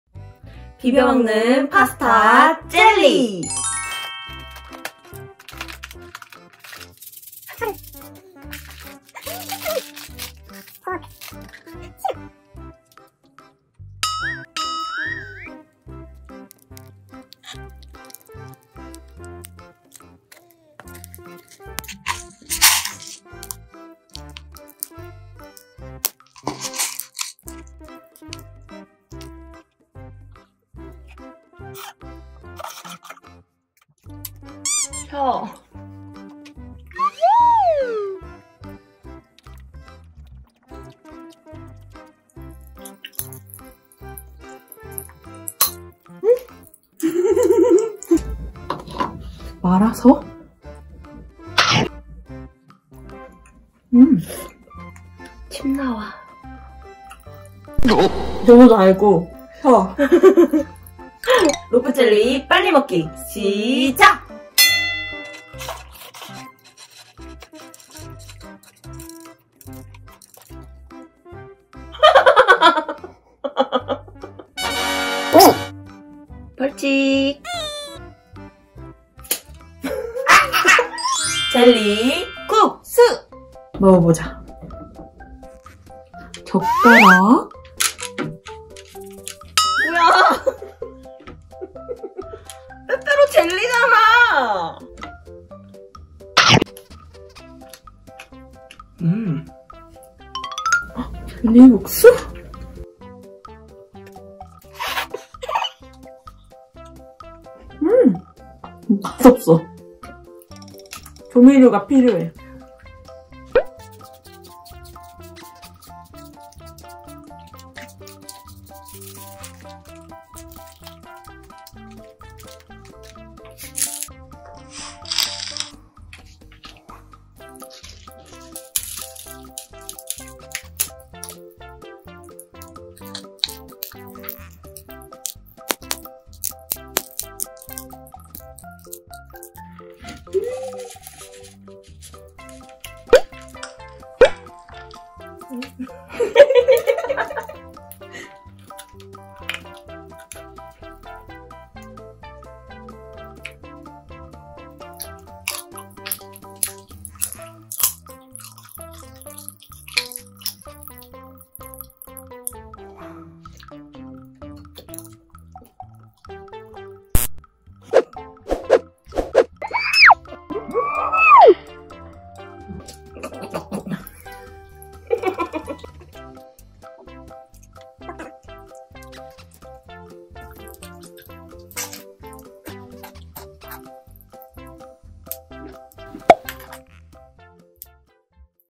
Jelly Noodles Party Rope Jelly Mukbang 비벼먹는 파스타 젤리 로프젤리 젤리국수파티 먹방 ASMR
영이진이의 신나는 젤리 국수 파티 !
Young and Jin's exciting jelly noodle party !

비벼먹는 파스타 젤리와 로프젤리 빨리 먹기 대결 ~
Mixed pasta jelly and rope jelly eating battle ~

승자는 누구 !?
Who's the winner !?

재미있게 보시고 같이 즐겨요 :P
Have fun and enjoy together :P